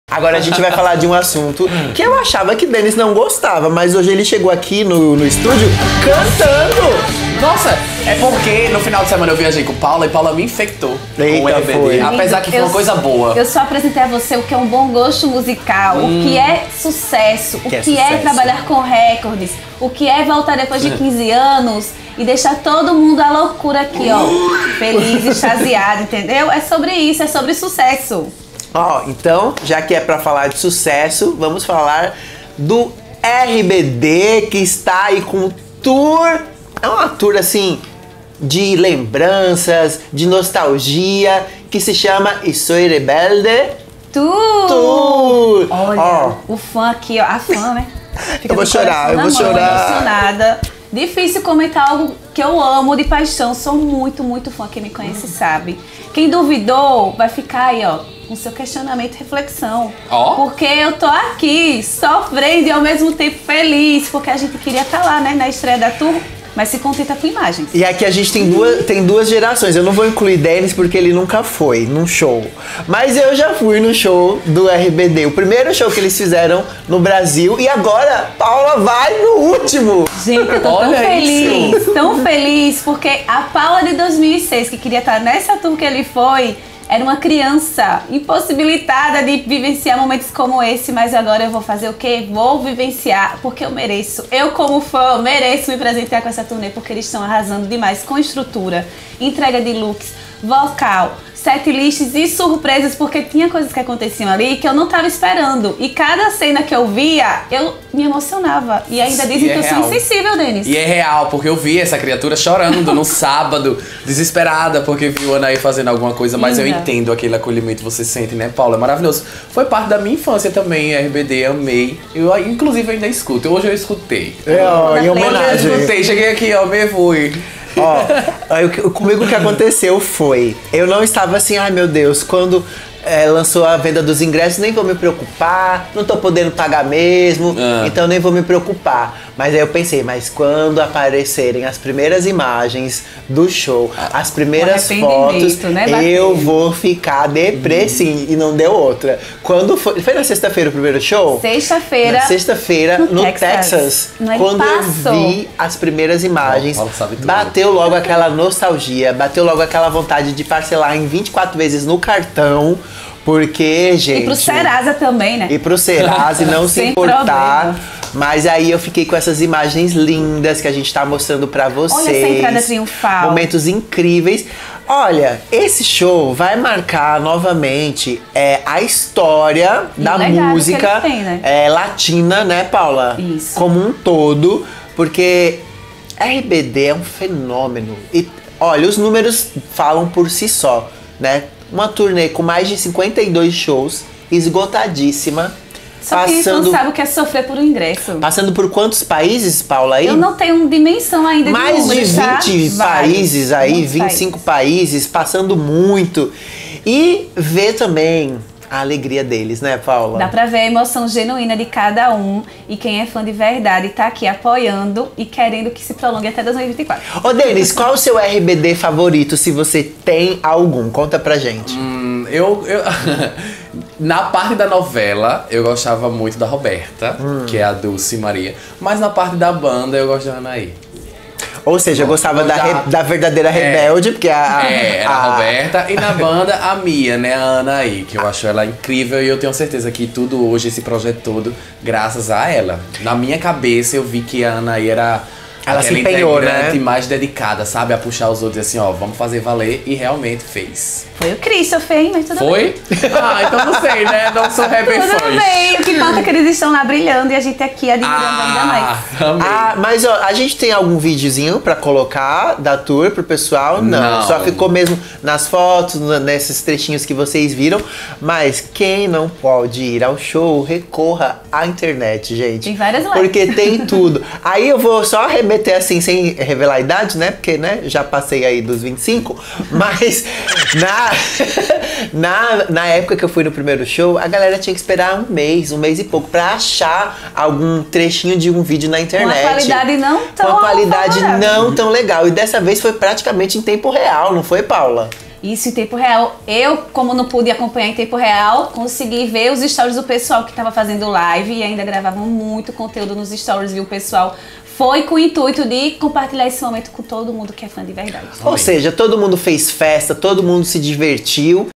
Grupo RBD inicia turnê mundial após 15 anos
• Instagram e Twitter: @redebtv